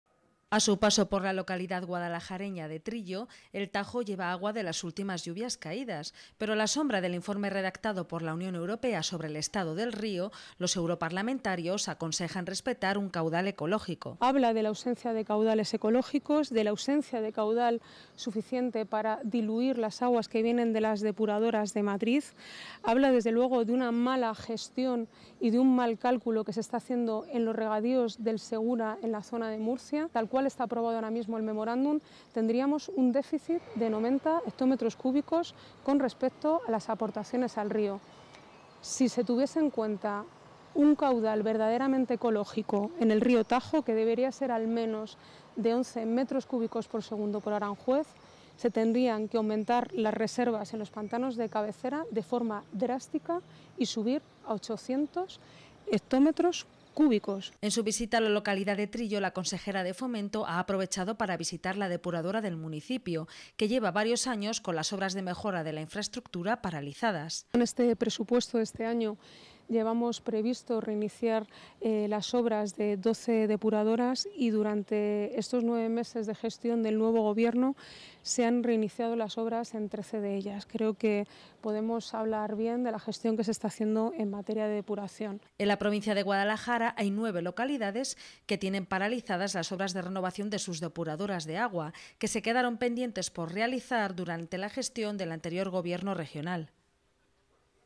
EL MEMORÁNDUM DEL TAJO PRESENTARÍA UN DÉFICIT DE 90 HECTÓMETROS CÚBICOS
El informe de los europarlamentarios sobre el Río Tajo sigue arrojando datos. La Consejera de Fomento asegura que este documento ratifica la postura del Gobierno regional  en materia de agua. De la Cruz asegura que el contenido del informe deja clara una mala gestión y cálculo de los regadíos del Segura en  Murcia y resalta que tal cual está aprobado el memorándum, estamos ante un déficit de 90 hectómetros cúbicos.